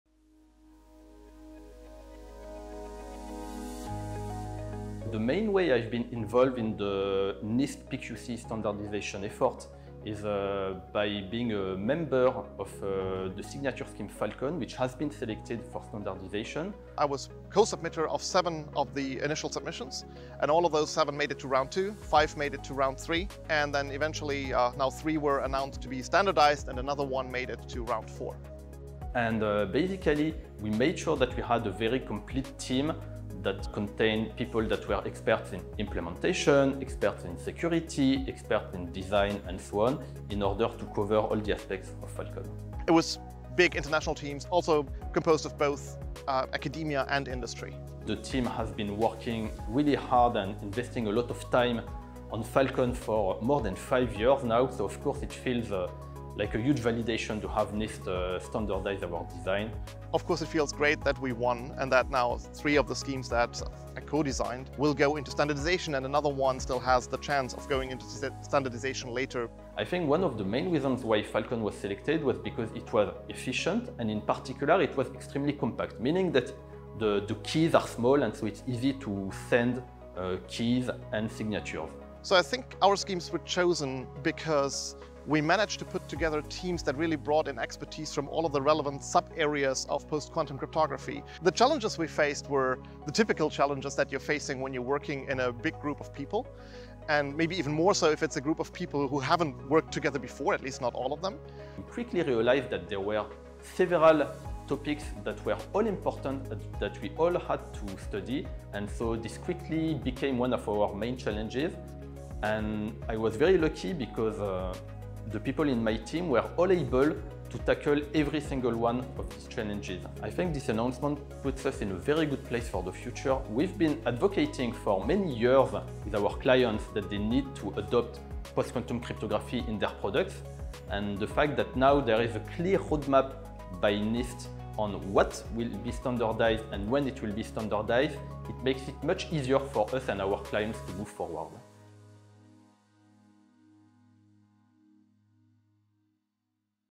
Team PQShield’s Role in the NIST Standards
Dr Thomas Prest and Prof Peter Schwabe: Curating the right team of experts, why keeping things simple is sometimes the best solution, and the future of post-quantum cryptography.